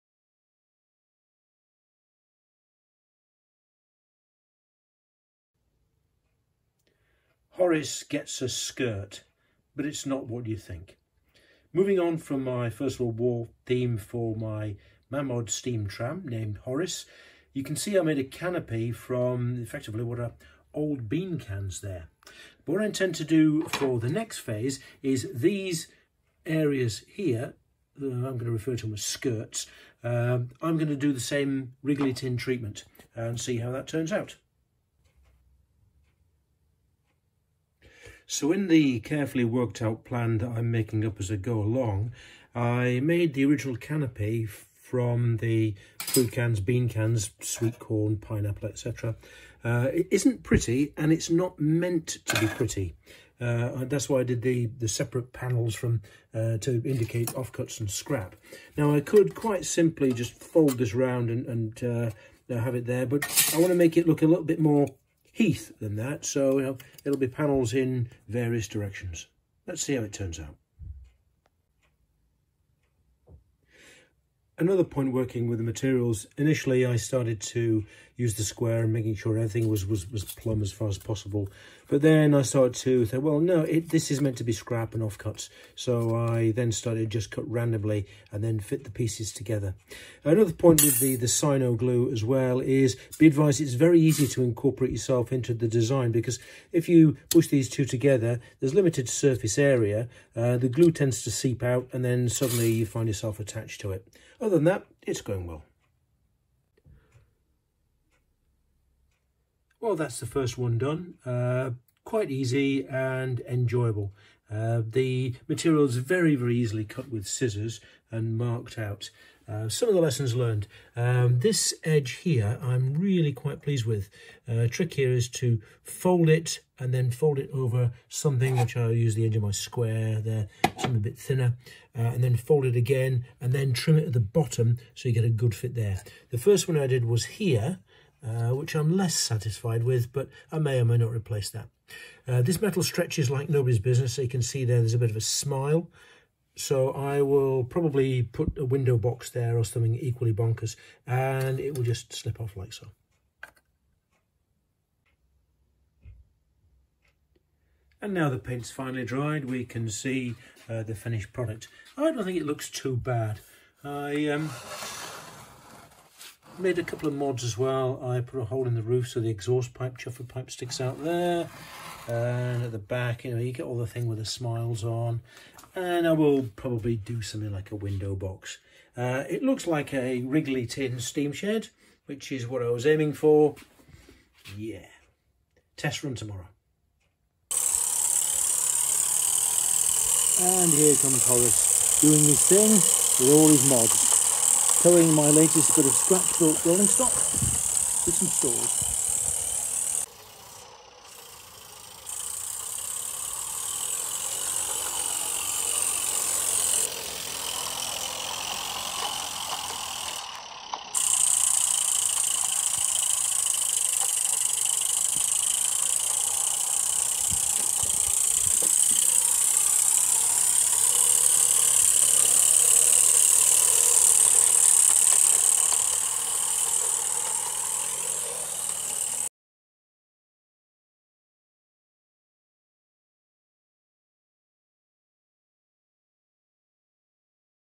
Horace gets a skirt
Continuing my WW1 theme, I made two skirts to fit on the frame of my Mamod Brunel loco 'Horace' from food tin cans.